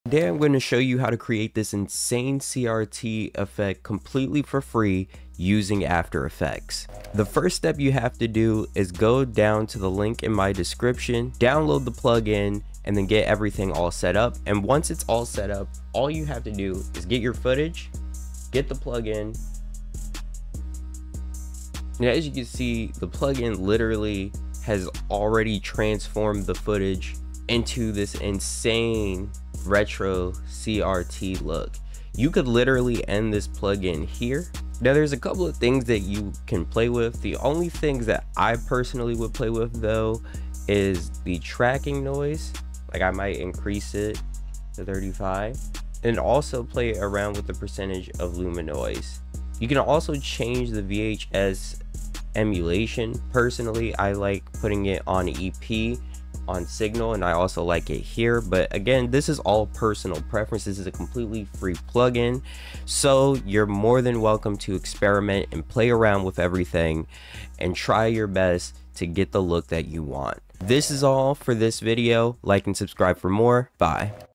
INSANE CRT PLUGIN THAT IS COMPLETELY FREE (AFTER EFFECTS)
In today's video you will learn how to create an insane CRT look with a completely free plugin!.